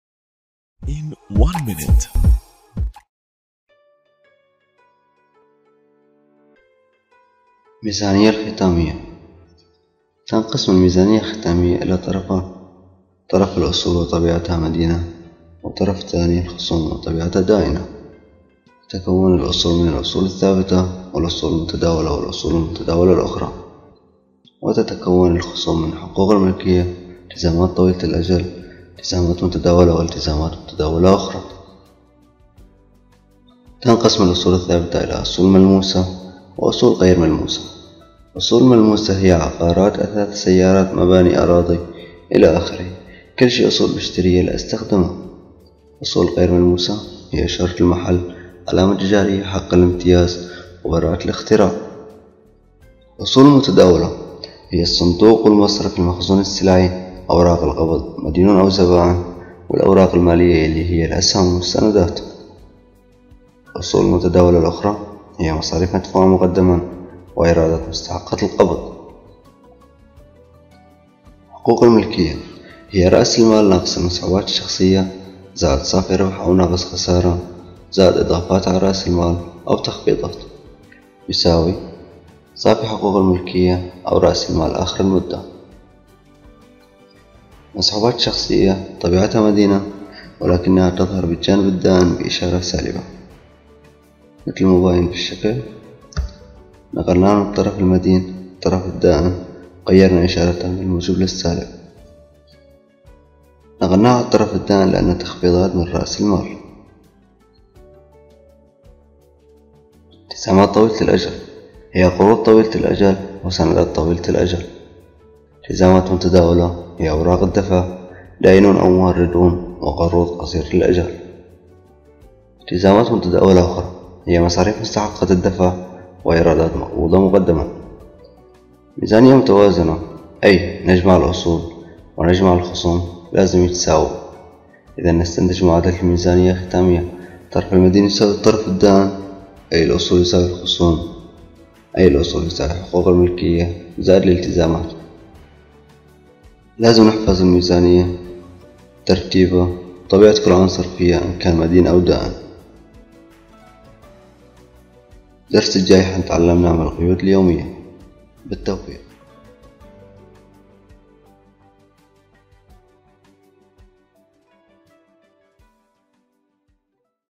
الميزانية العمومية || Balance sheet
الهدف من إعداد الميزانية العمومية هو معرفة وضع الشركة المالي وكيفية وضعها في السوق من ناحية قوة استمرارها  من حيث إن أصولها تغطي التزاماتها دون النقص والتأثير على رأس المال لأن معادلة الميزانية = الأصول مطروح منها الخصوم = حقوق ملكية  وهذا يعني أن قدرة الشركة على سداد التزاماتها عن طريق أصولها دون التأثير على حقوق الملكية هكذا تكون الشركة بالوضع السليم بالسوق
المراجع References:
مبادئ المحاسبة -كلية الاقتصاد-جامعة دمشق
هذه الدورة من اعداد الطالب:
جمعة الشبلي
من دورة I want to become a professor